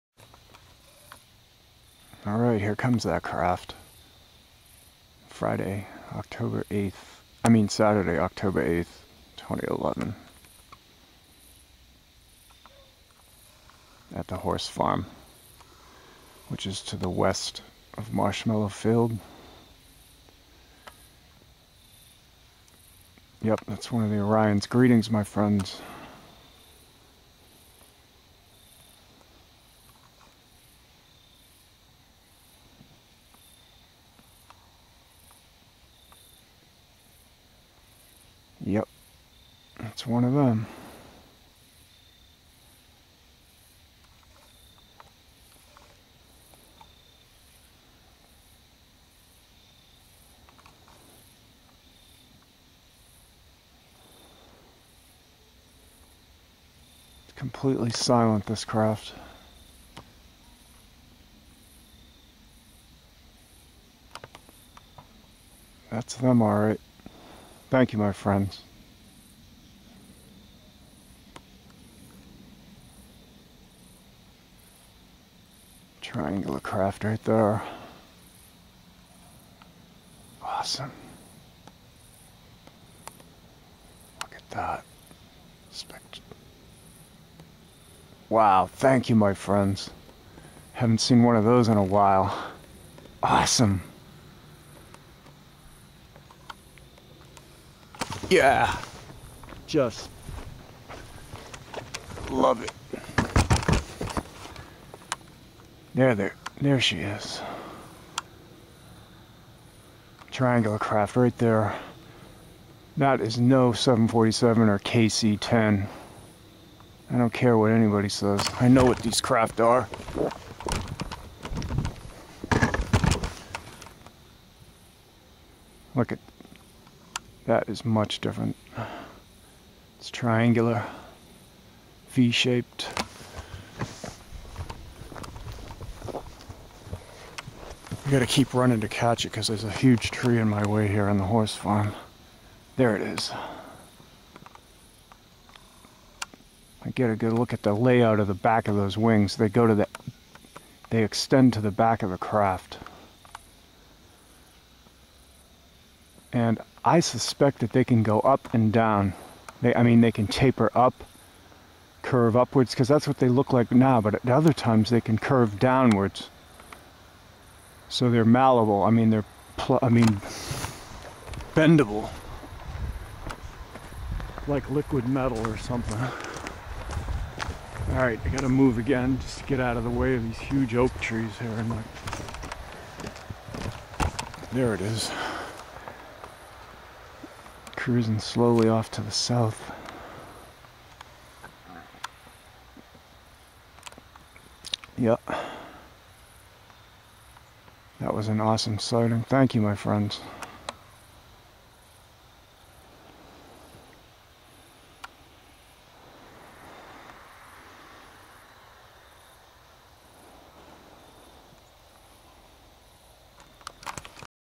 Triangular Craft- Awesome New Sighting-Oct-8-2011
Really awesome sighting of one of the Oxicarian ships powering up it's light
for me as I greet them, Saturday evening Oct-8-2011  location Orange county NY.